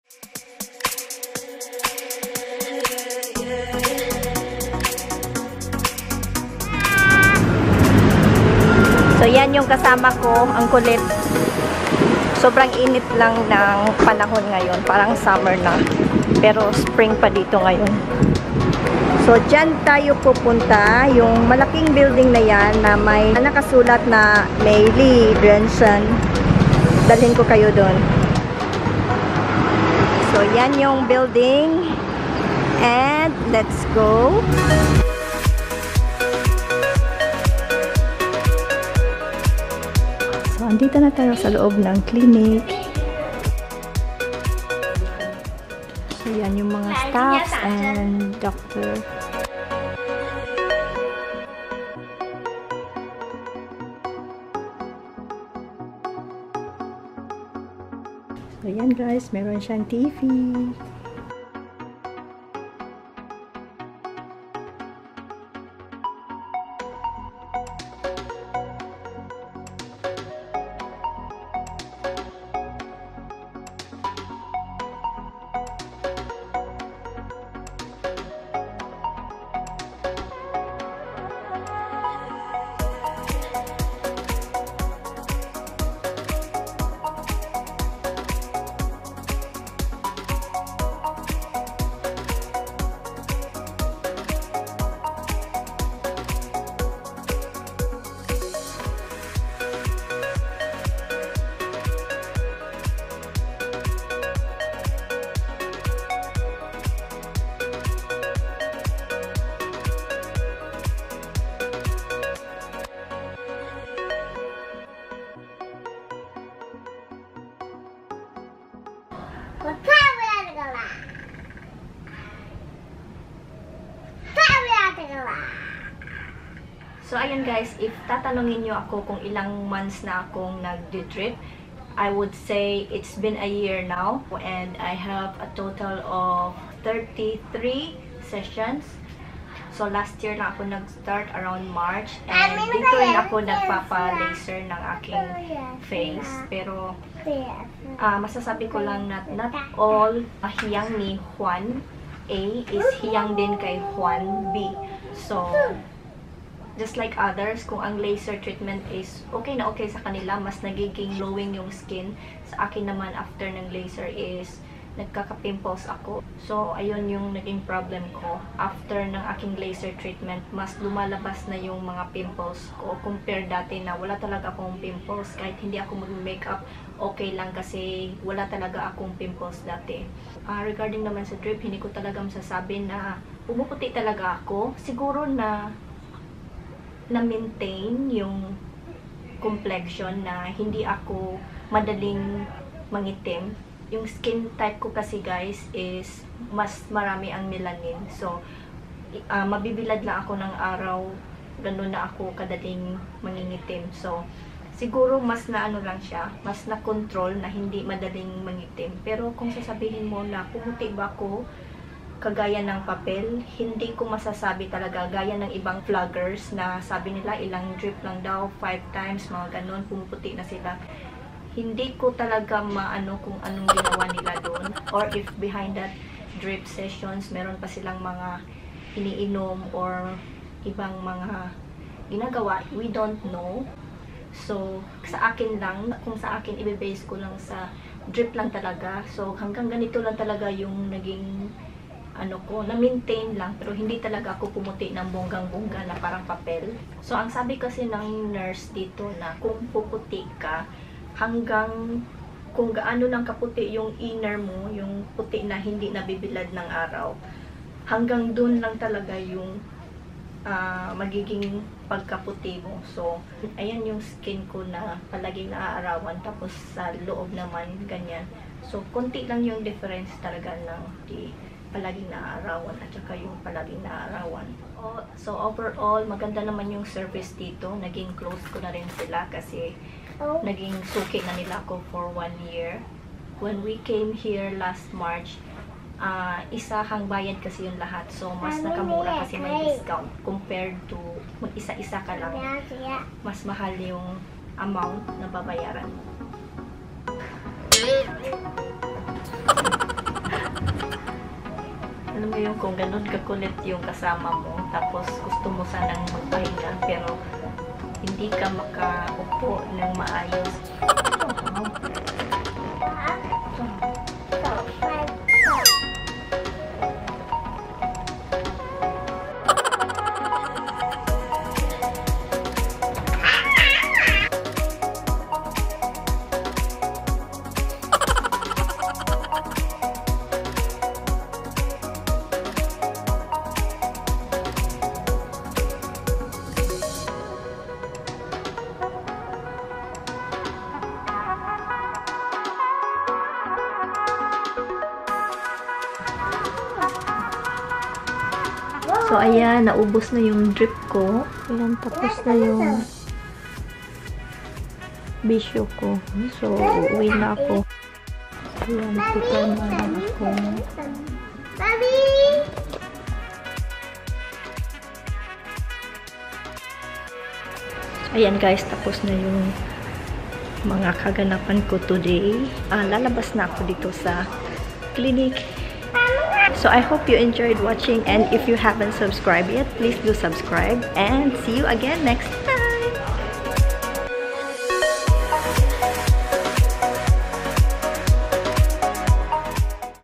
GLUTA DRIP | PUMUTI BA AKO? | AN KLEH HONEST REVIEW
Hi guys! It's me again.
And for today's episode, I'll share with you my beauty drip IV glutathione experience here in Taiwan. It's actually my 26th session this time (out of 33 sessions) since March last year. 😊

I had my ND Yag laser on my face to remove my freckles, but the result is , i had a pimple break out after the laser treatment. I have a total of 8 sessions for ND Yag laser and the second to the last treatment went well compared to the first 6 sessions and  I had fewer pimples this time.

If you have some inquiries about the aesthetic clinic I consulted, or you want to know the other services they offer, you can just walk in to their clinic or make a call:

地址：新北市三重區光復路一段45號
No. 45, Section 1, Guangfu Rd., Sanchong District, New Taipei City.

We will show you more of Taiwan in our upcoming Taiwanderful Episodes! 😍

Mwah! Love yah'all!